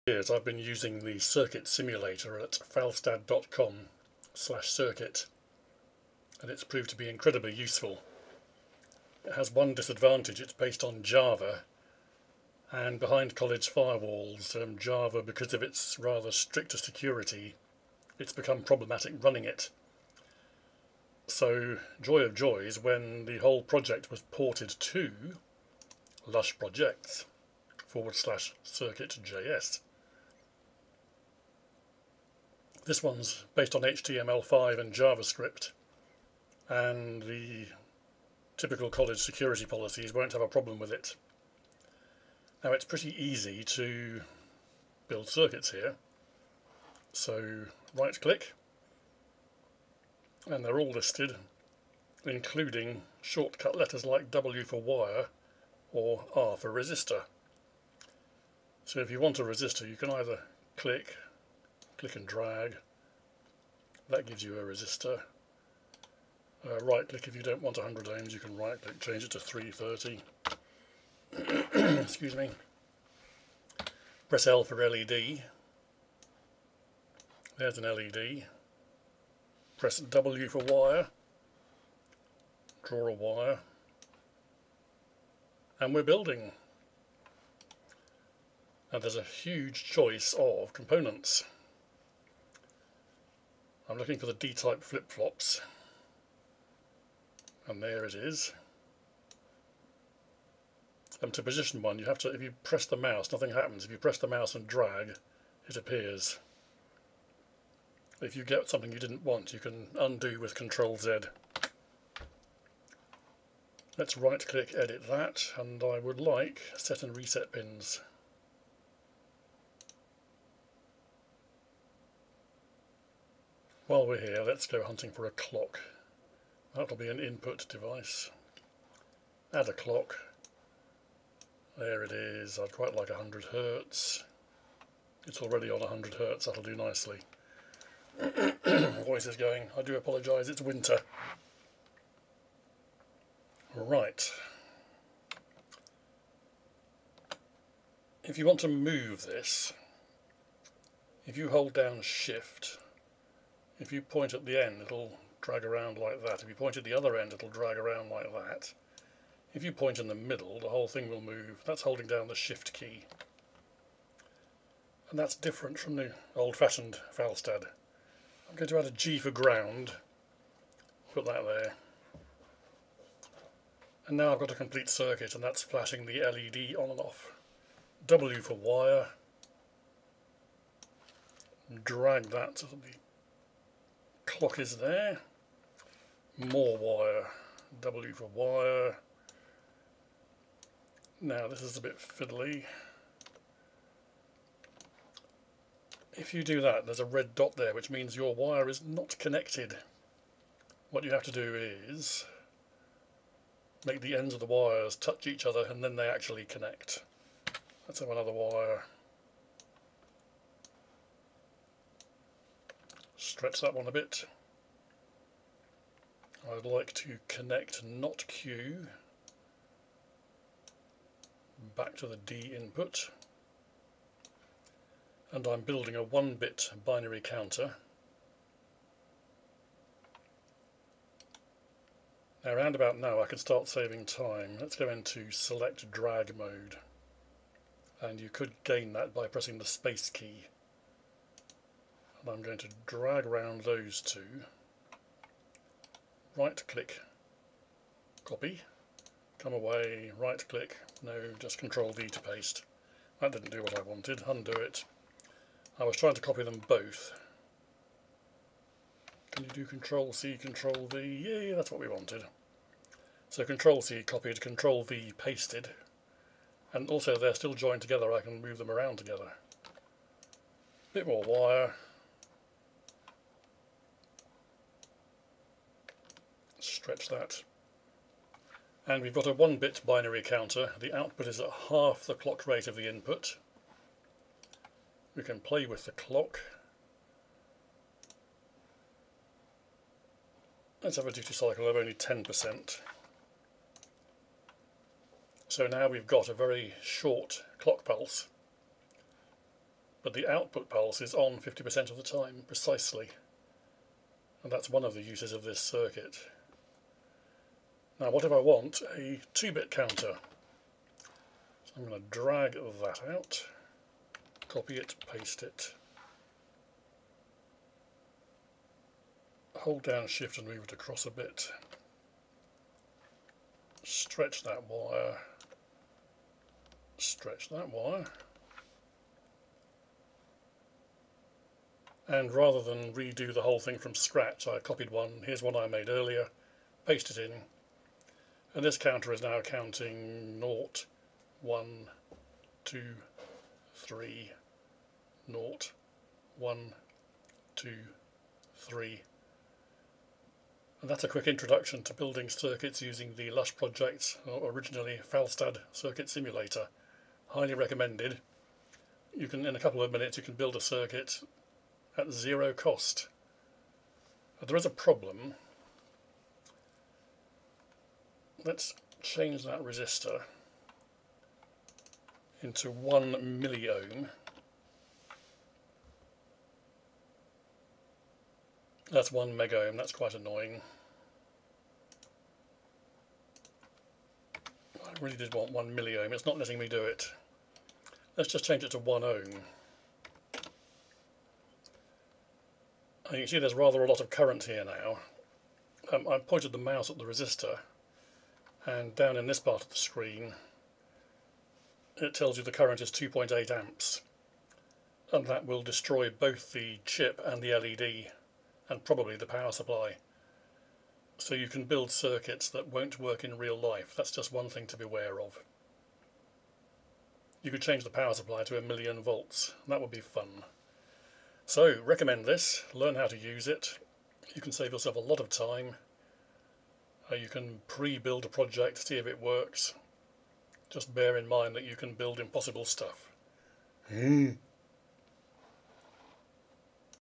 LushProjects Falstad Simulator
Lushprojects is a JavaScript port of the old faithful Falstad simulator. This is a video guide to using the simulator. The first couple seconds of audio got chopped off but nothing important is missing. The video shows a complete circuit being built but oscilloscopes were not demonstrated.